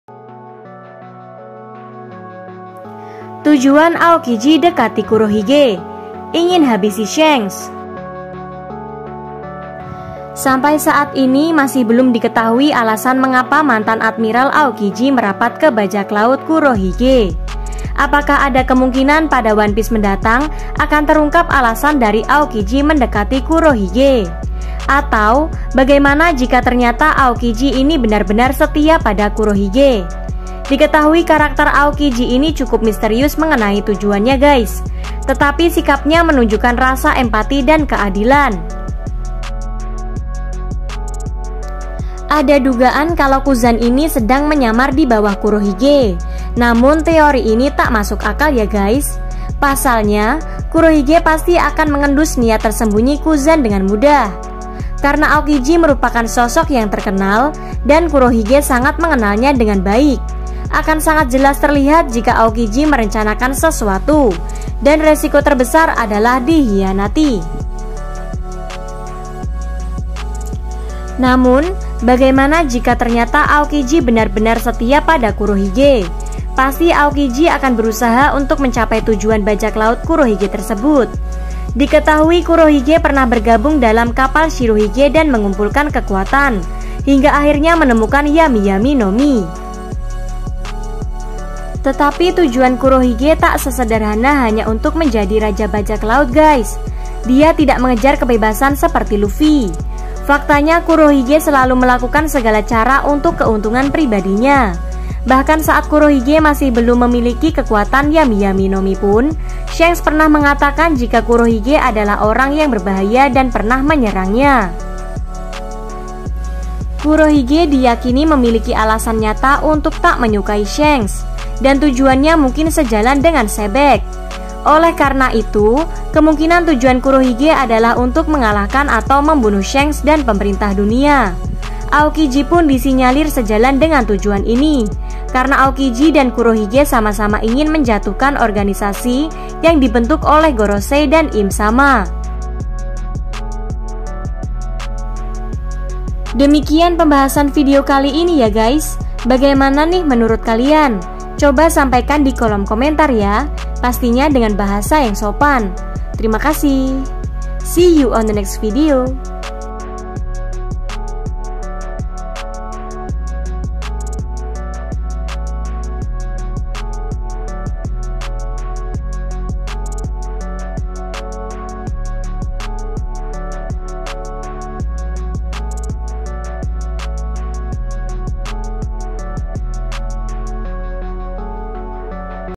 One Piece Terbaru - Tujuan Aokiji Dekati Kurohige, Ingin Habisi Shanks
Tujuan Aokiji Dekati Kurohige, Ingin Habisi Shanks

Sampai saat ini, masih belum diketahui alasan mengapa mantan Admiral Aokiji merapat ke Bajak Laut Kurohige.
Apakah ada kemungkinan pada One Piece mendatang akan terungkap alasan dari Aokiji mendekati Kurohige?
Atau, bagaimana jika ternyata Aokiji ini benar-benar setia pada Kurohige.
Diketahui karakter Aokiji ini cukup misterius mengenai tujuannya,
tetapi sikapnya menunjukkan rasa empati dan keadilan.

Ada dugaan kalau Kuzan ini sedang menyamar di bawah Kurohige, namun teori ini tak masuk akal.
Pasalnya, Kurohige pasti akan mengendus niat tersembunyi Kuzan dengan mudah.
Karena Aokiji merupakan sosok yang terkenal, dan Kurohige sangat mengenalnya dengan baik,
Akan sangat jelas terlihat jika Aokiji merencanakan sesuatu, dan resiko terbesar adalah dikhianati.

Namun, bagaimana jika ternyata Aokiji benar-benar setia pada Kurohige,
Pasti Aokiji akan berusaha untuk mencapai tujuan bajak laut Kurohige tersebut.
Diketahui Kurohige pernah bergabung dalam kapal Shirohige dan mengumpulkan kekuatan,
hingga akhirnya menemukan Yami Yami no Mi. Tetapi tujuan Kurohige tak sesederhana hanya untuk menjadi Raja Bajak Laut,
dan dia tidak mengejar kebebasan seperti Luffy.
Faktanya Kurohige slalu melakukan segala cara untuk keuntungan pribadinya.
Bahkan saat Kurohige masih belum memiliki kekuatan Yami Yami no Mi pun,
Shanks pernah mengatakan jika Kurohige adalah orang yang berbahaya, dan pernah menyerangnya.

Kurohige diyakini memiliki alasan nyata untuk tak menyukai Shanks,
dan tujuannya mungkin sejalan dengan Xebec.
Oleh karena itu, kemungkinan tujuan Kurohige adalah untuk mengalahkan atau membunuh Shanks dan Pemerintah Dunia.
Aokiji pun disinyalir sejalan dengan tujuan ini,
karena Aokiji dan Kurohige sama sama ingin menjatuhkan organisasi yang dibentuk oleh Gorosei dan Im-sama.

Demikian pembahasan video kali ini,
Bagaimana menurut kalian? sampaikan di kolom komentar ya?

Dan yang paling penting, aktifkan tombol notifikasi agar selalu update dengan video terbaru